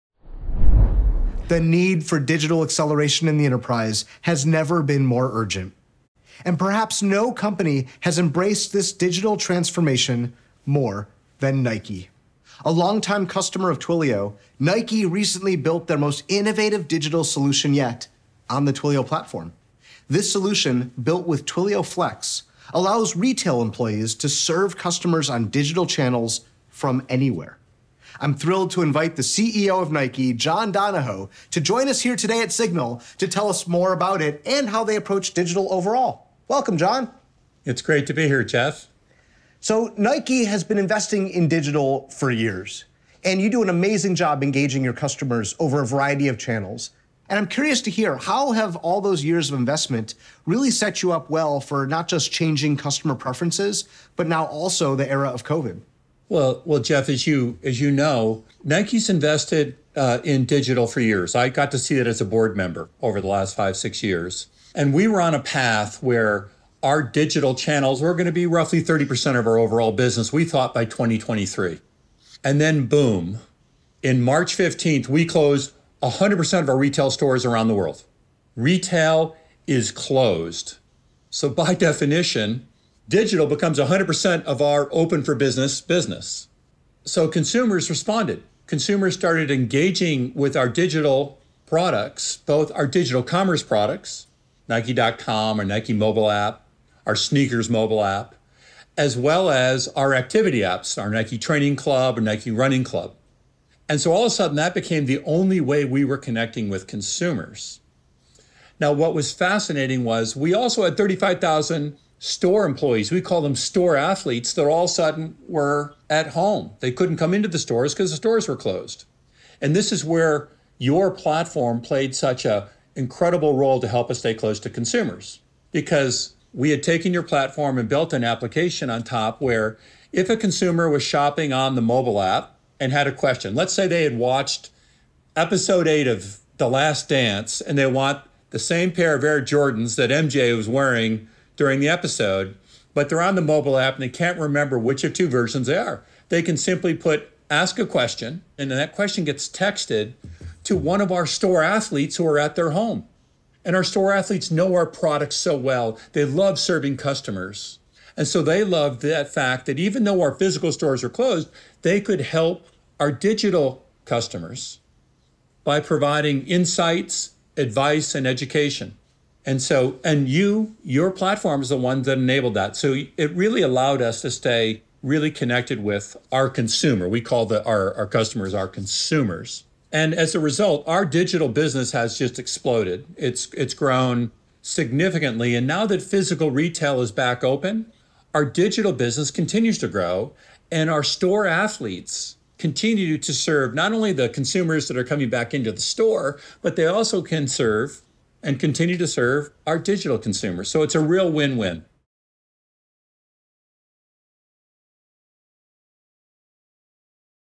Nike CEO shares how Twilio helped with their digital acceleration
“Retail is closed. By definition, digital becomes 100 percent of our open-for-business business.” -Nike CEO John Donahoe

How did one of the world’s digital retail leaders push innovation even further when faced with the challenges of the COVID-19 pandemic? Through personalized service, customer-driven channels, and keen understanding of how their savvy customers want to communicate. With Twilio Flex, Nike transformed their 35,000 in-store ‘athlete’ customer support agents into a digital fleet of knowledgeable customer engagement specialists. Watch Nike CEO John Donahoe run through how digital athletes help consumers find exactly the right shoe, answer any question, and build relationships that have continued to thrive as physical retail safely reopens.